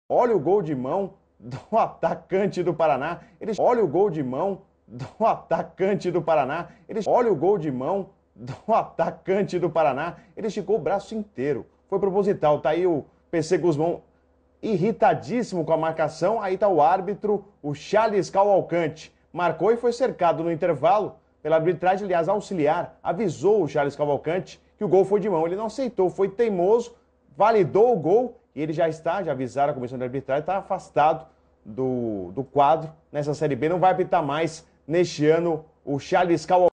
Lindo GOL de Mão na Série B - IG
Série B: veja um incrível gol de mão
Gol um gol de mão validado pelo árbitro, o Paraná derrotou o Ceará por 1 a 0.